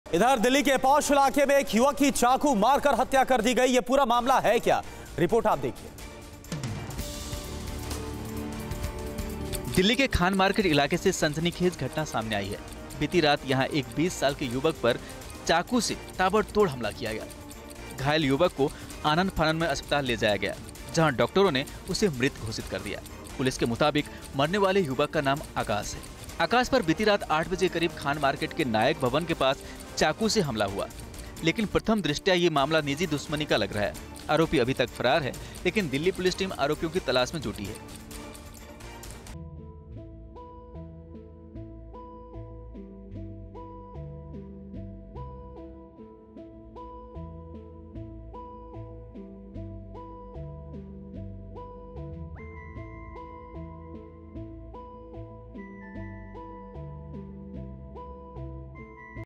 Khan Market Murder: दिल्ली के खान मार्केट में 20 वर्षीय युवक पर चाकू से ताबड़तोड़ हमला
दिल्ली के खान मार्केट इलाके से एक सनसनीखेज घटना सामने आई हैं, जहां एक  20 वर्षीय युवक पर चाकू से हमला किया गया, पीड़ित को आनन-फानन में अस्पताल ले जाया गया जहां डॉक्टरों ने उसे मृत घोषित कर दिया. दिल्ली पुलिस ने यह जानकारी दी है.